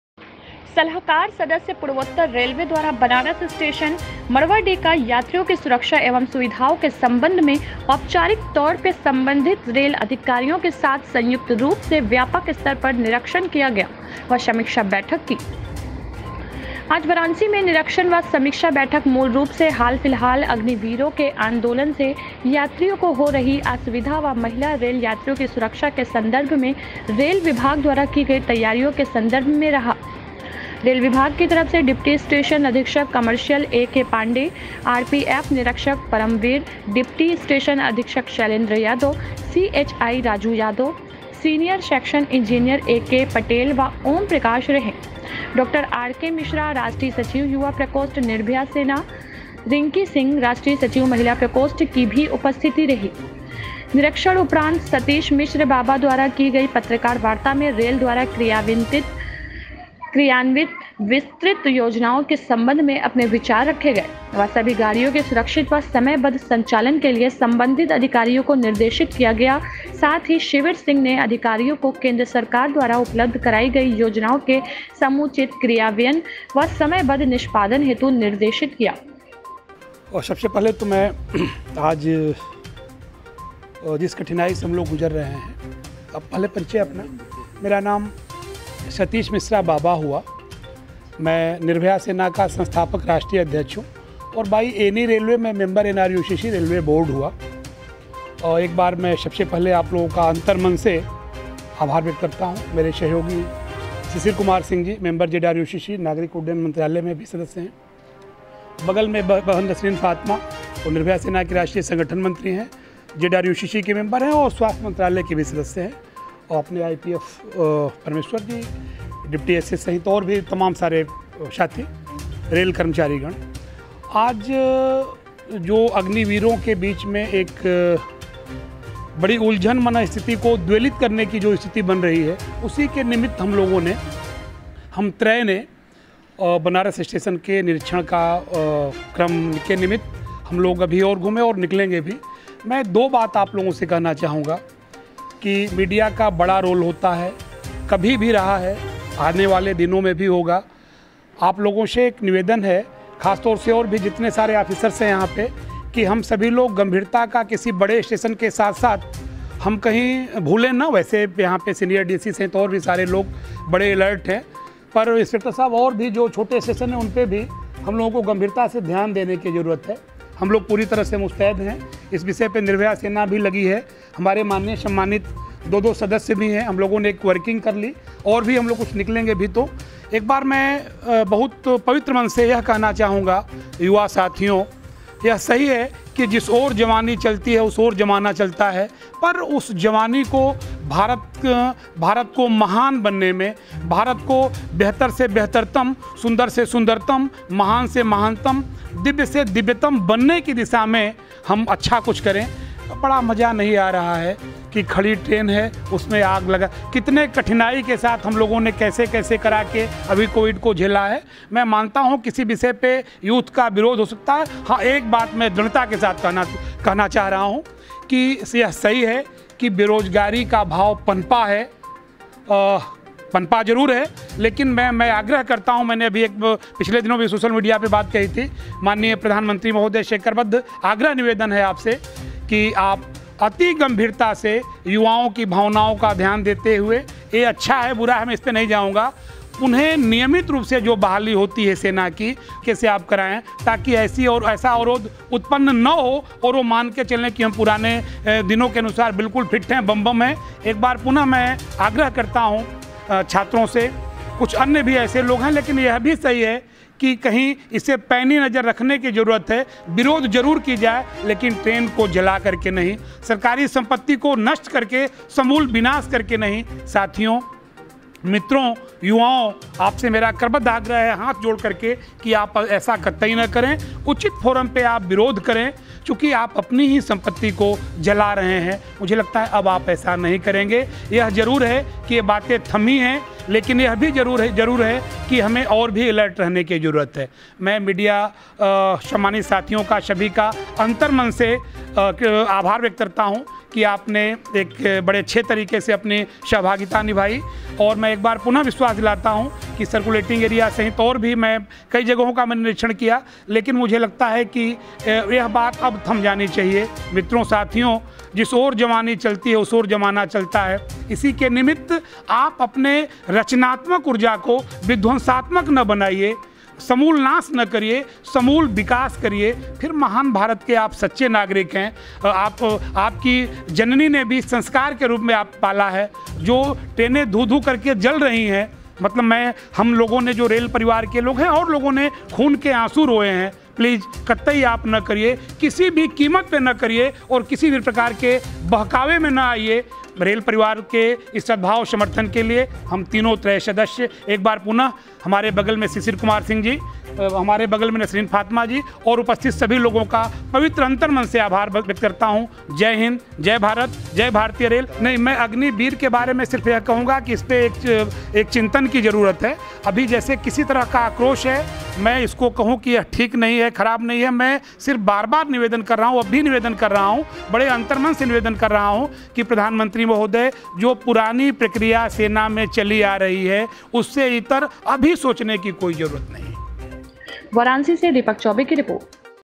सलाहकार सदस्य, पूर्वोत्तर रेलवे द्वारा बनारस स्टेशन का निरीक्षण किया गया व समीक्षा बैठक की
सलाहकार सदस्य, पूर्वोत्तर रेलवे द्वारा बनारस स्टेशन (मंडुआडीह) का यात्रियों की सुरक्षा, एवं सुविधाओं के संबंध में औपचारिक तौर पे संबंधित रेल अधिकारियों के साथ संयुक्त रूप से व्यापक स्तर पर निरीक्षण किया गया व समीक्षा बैठक की